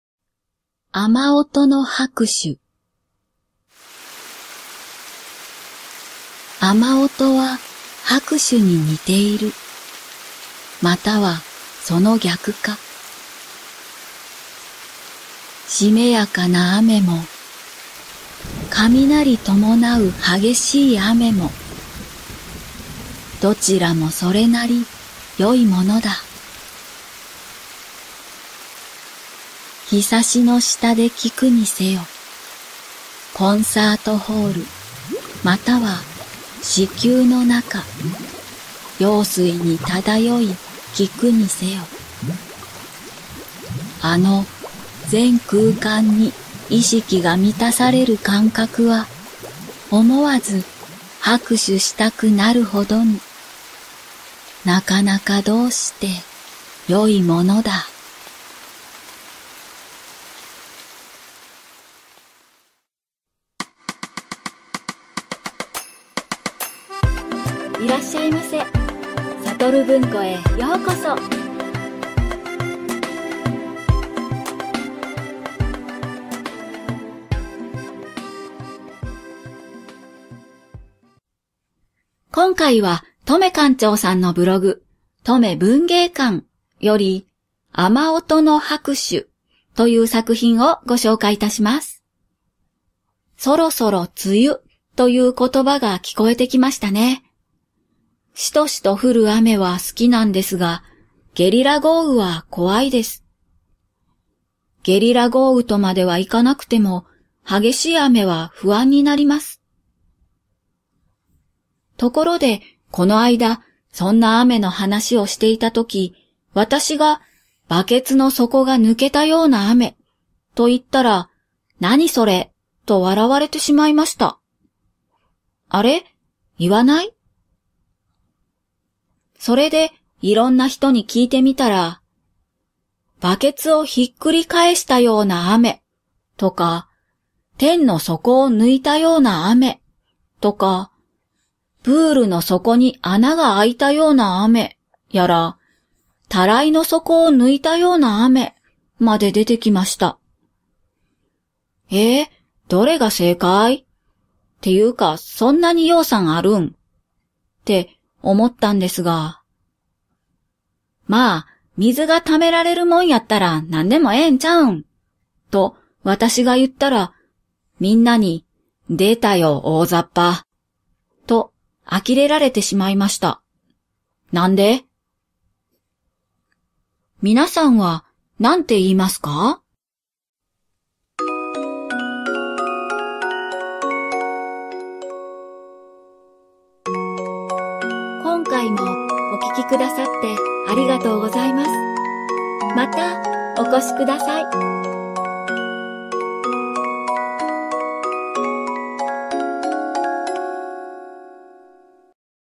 さとる文庫 2号館 第478回 雨音の拍手
声の出演･脚本･音響：もぐら　原作･映像：Tome館長

「さとる文庫 2号館」もぐらさんが演じてくださった！

　その他

雨音は　拍手に似ている。
　または　その逆か。

しめやかな雨も
　雷ともなう　激しい雨も

　どちらも　それなり
　　よいものだ。

ひさしの下で　聞くにせよ

コンサートホール　または
　子宮の中　羊水に漂い　聞くにせよ

　あの全空間に意識が満たされる感覚は
　　思わず拍手したくなるほどに

　なかなか　どうして
　　よいものだ。

[ Applause of the Sound of Rain ]

The sound of rain is similar to applause.
Or the opposite?

Also moist rain
Heavy rain with thunder

Both are good things.

Listen under the eaves

In the concert hall or
Drift in amniotic fluid in the womb

The sense that
consciousness is filled in that whole space is
Involuntarily want to applause

What a good thing.